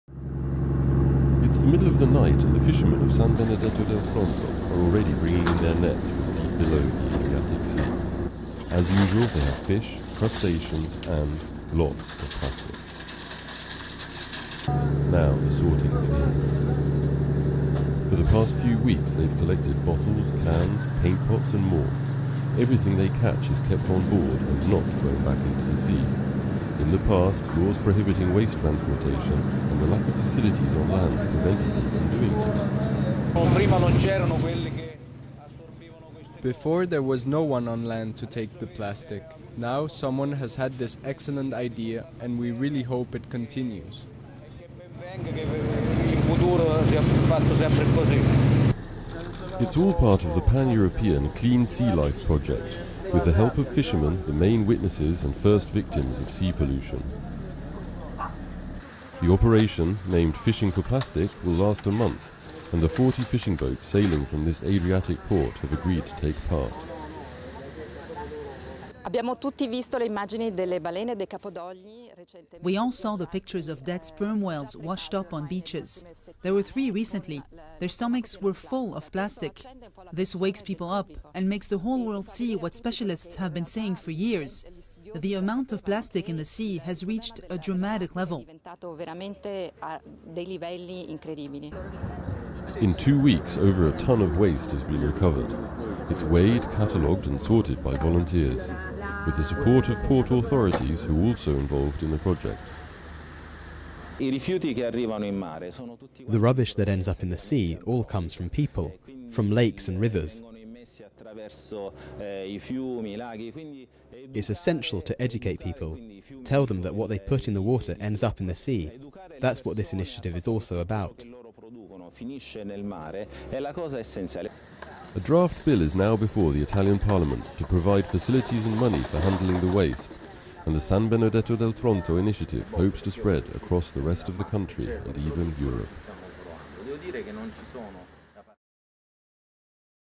Italian fishermen fight to rid the sea of plastic waste
Over the last few weeks several sperm whales have washed up on Italy's beaches, their stomachs full of plastic. Sea plastic is threatening the marine ecosystem, with fishermen on the frontline. At San Benedetto del Tronto, they're taking part in "fishing for plastic", a month-long experiment to get them to bring the plastic waste they catch in their nets back to dry land instead of throwing the rubbish back into the water. (AFP)